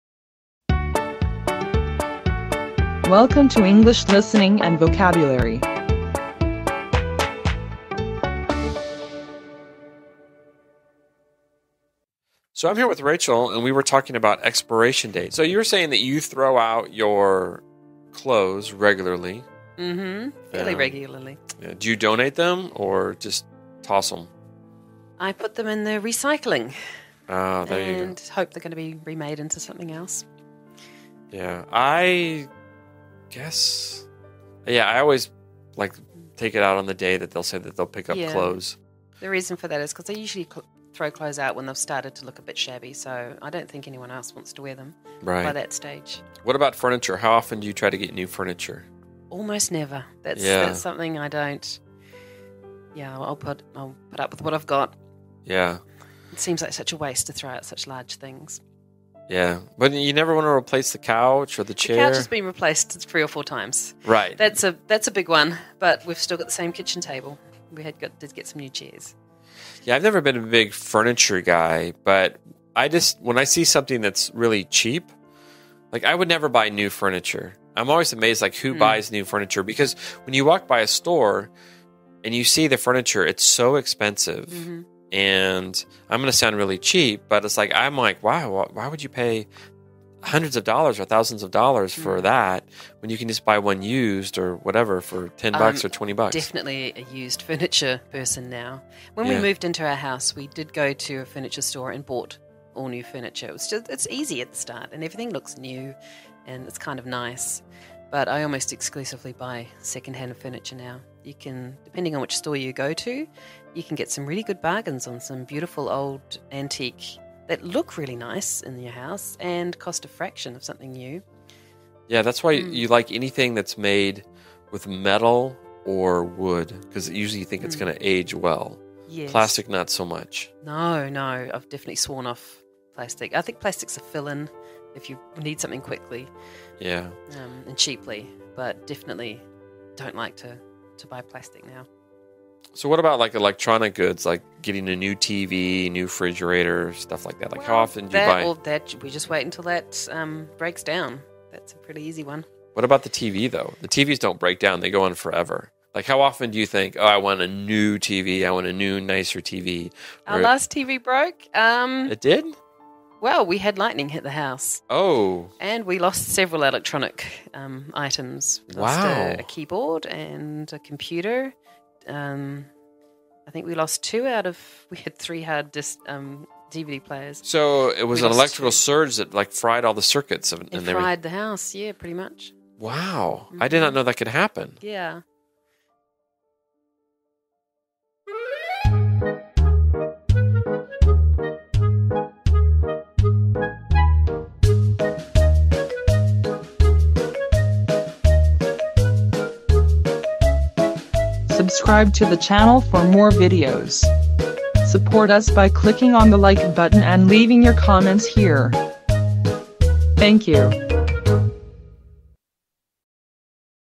Throwing Things Out 👉 English Listening and Vocabulary ✅ Level 6
A short story helps to improve your English Vocabulary and Listening Skills.
Used images are from unsplash.com, and pixabay.com.